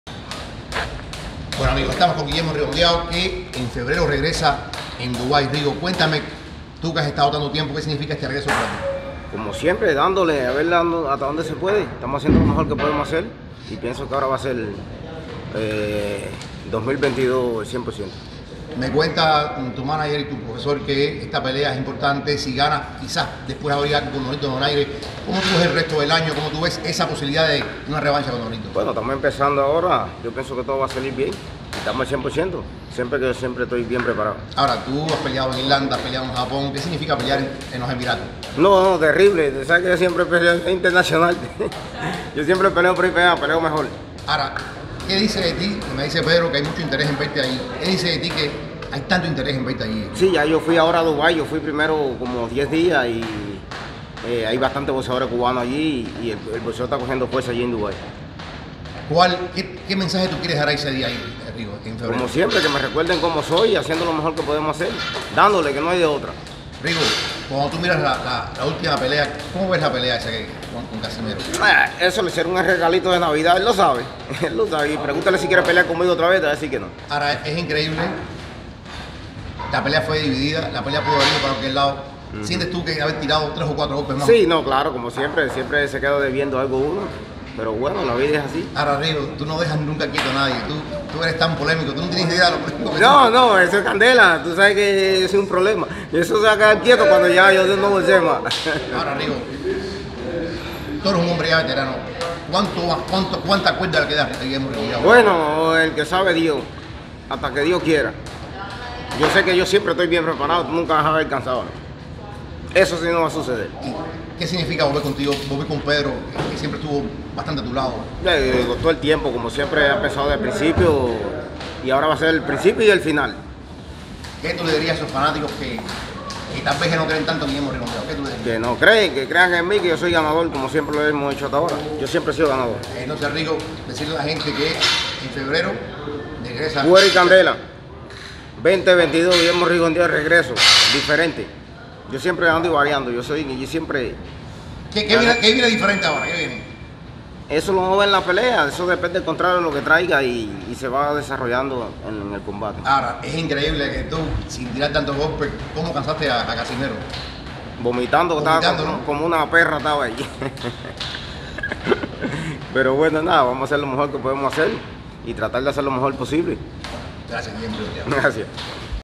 GUILLERMO RIGONDEAUX: A Casimero le hicieron un regalito de Navidad
La brújula de Guillermo Rigondeaux parece llevarlo por un nuevo rumbo pero a otro rival filipino. El cubano parte este lunes a enfrentar a Vincent “Acero’‘ Astrolabio en su regreso al cuadrilátero el próximo 26 de febrero en Dubai, como parte de una serie de carteleras de boxeo en los Emiratos Árabes Unidos.
Rigondeaux tuvo su mejor momento cuando superó en el 2013 a un inmenso Nonito Donaire y hace unos meses cayó por decisión dividida frente a John Riel Casimero, pero en ese proceso hizo lucir muy mal al también filipino, quien en algún momento tuvo que vomitar en su esquina debido al cansancio.
Ahora enfrenta a su cuarto guerrero -el otro es Drian Francisco- nacido en ese archipiélago asiático, de la mano del profesor Pedro Díaz, la cabeza de Mundo Boxing, quien ya estuvo en su esquina para el inolvidable choque frente a Donaire. Ahora intentará escribir un nuevo capítulo en su carrera, cuando todo parecía acabado.